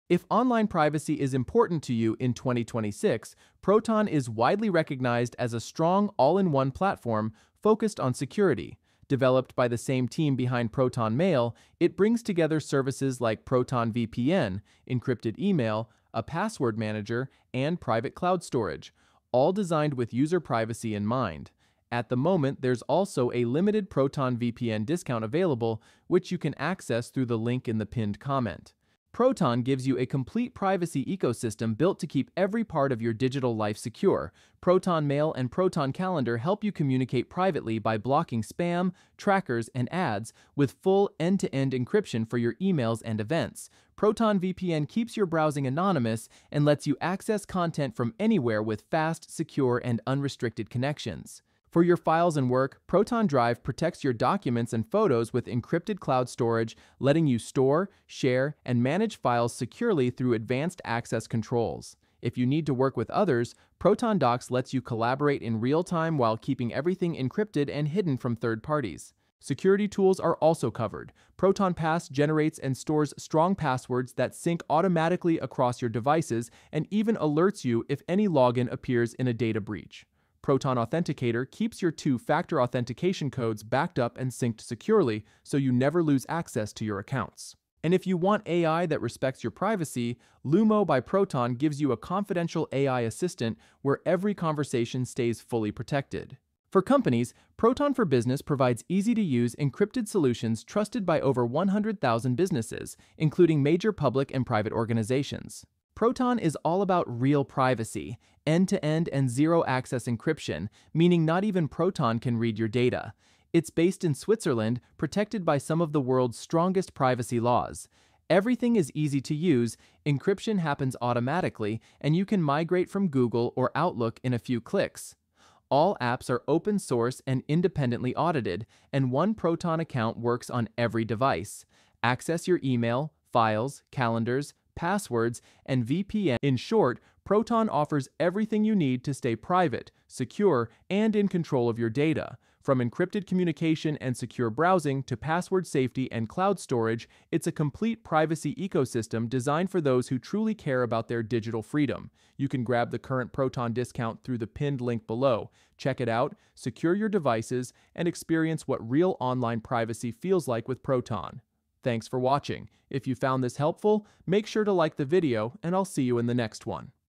Ultimate Proton Privacy Review 2026 - Proton VPN, Proton Mail, Proton Pass + Discount Code
Short review of the full Proton privacy suite in 2026, covering Proton VPN, Proton Mail, and Proton Pass, plus how to get the best available discount code!